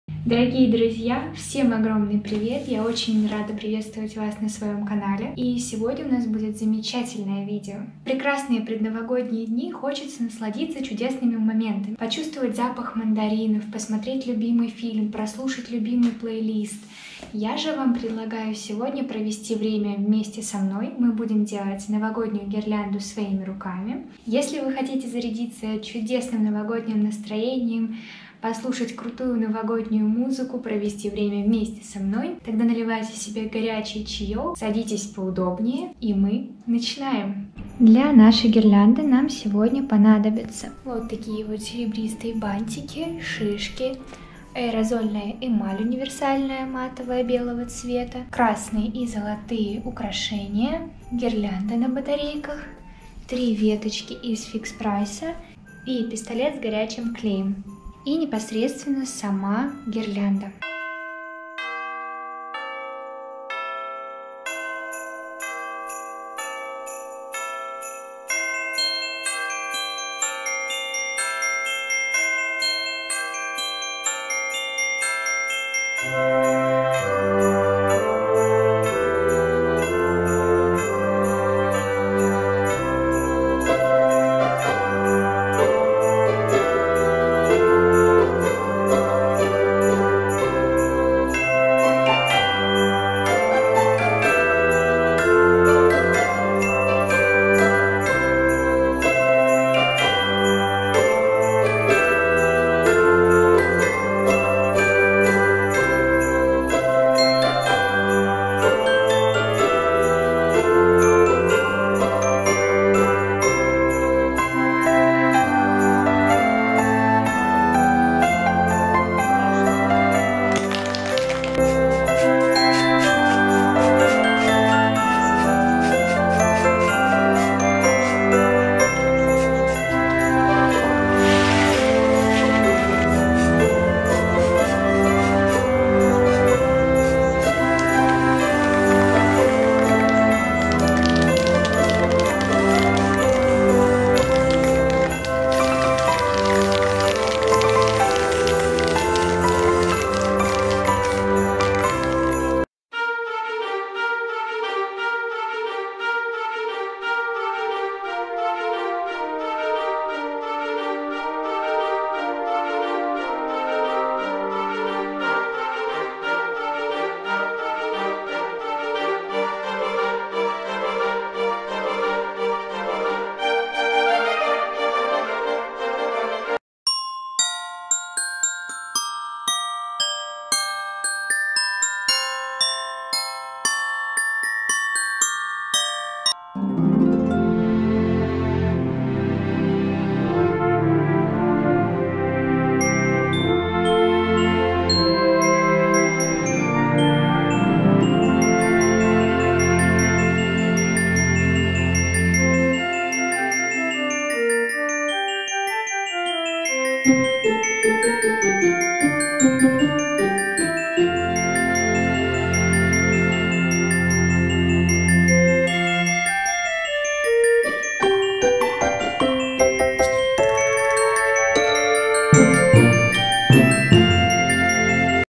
DIY Новый год и Рождество/Создаем Новогоднюю Гирлянду
Всем огромный привет!❤️
Если вдруг у вас были мысли дополнить украшениями ваш дом, но Вы не знали что именно добавить, то это видео непременно для Вас!
В этом видео я  делаю новогоднюю гирлянду вместе с Вами с нуля!
Все, что мне понадобится вы удивите в самом ролике, не буду много расписывать, просто смотрите с удовольствием!
И всех с Новым 2021 годом🎁🎄